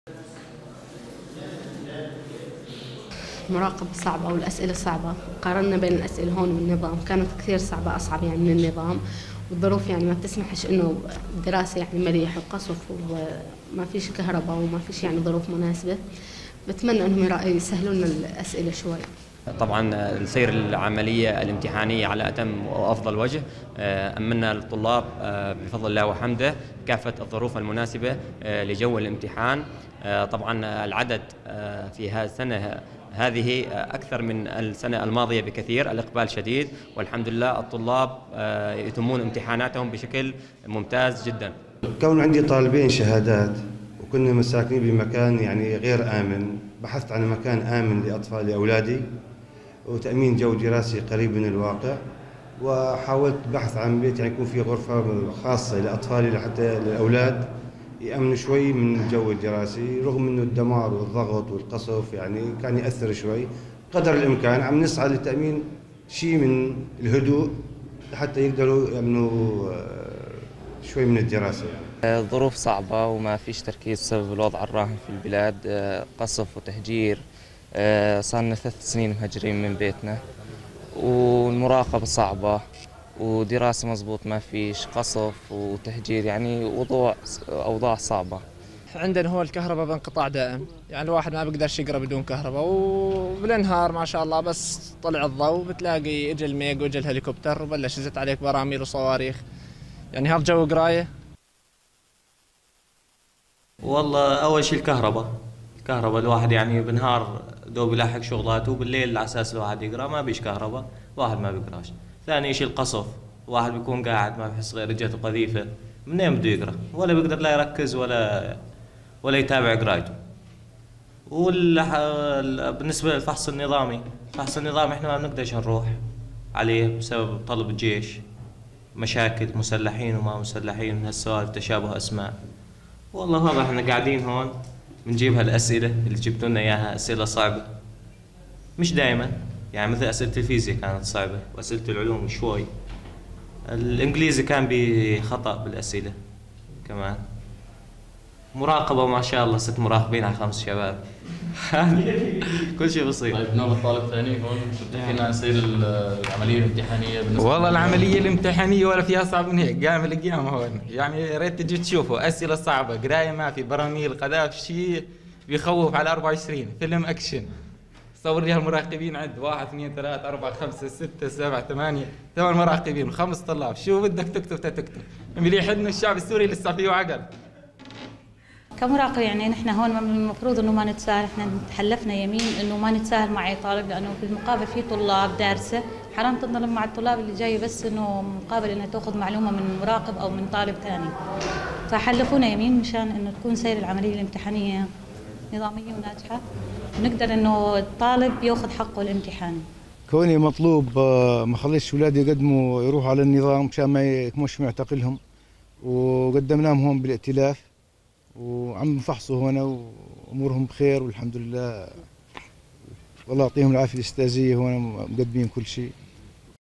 أورينت نيوز تقف على أحوال الطلاب في مراكز درعا الامتحانية
صعوباتٌ كثيرة تواجهُ طلابَ درعا الذينَ يتقدمونَ في هذه الأيام لعلَّ أهمَها
القصفُ والتهجيرُ وانقطاعُ الكهرباء .. بشر أحمد زارَ عدداً من مراكزِ
الامتحانات ووقفَ على آراءِ الطلاب والأهالي
بشر أحمد – مراسل أورينت نيوز – درعا